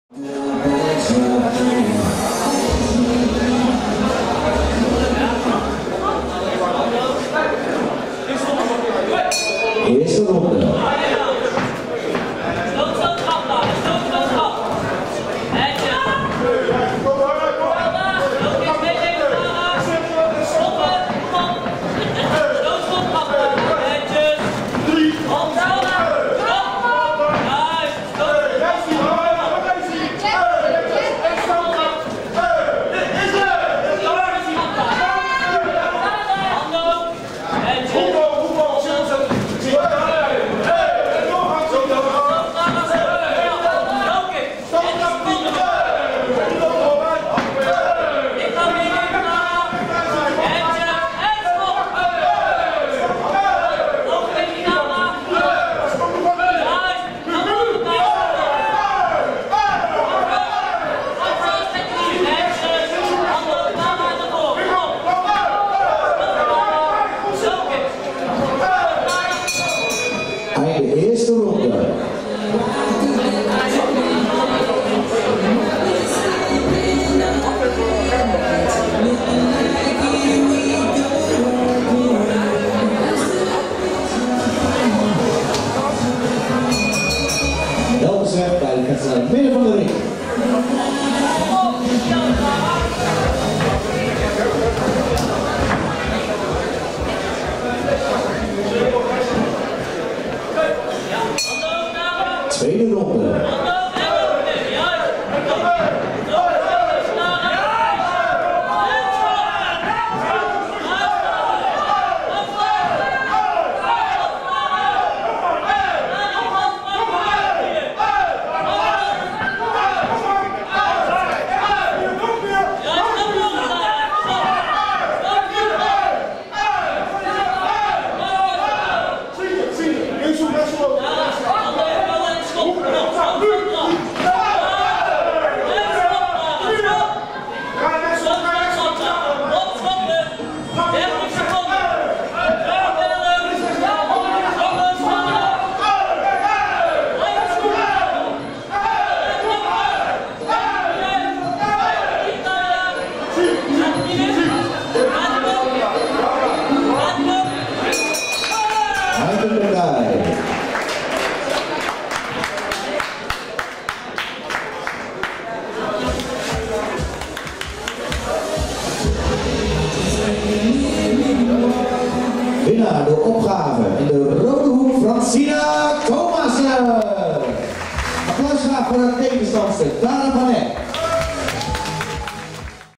Francina Tomassen vs Tara van Eck
Junior fight Muay Thai Francina Tomassen (MTC van Dams) vs Tara van Eck (Budokai Breda) @ Muay Thai event Elsomo Gym, Kruisweg Marum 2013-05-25.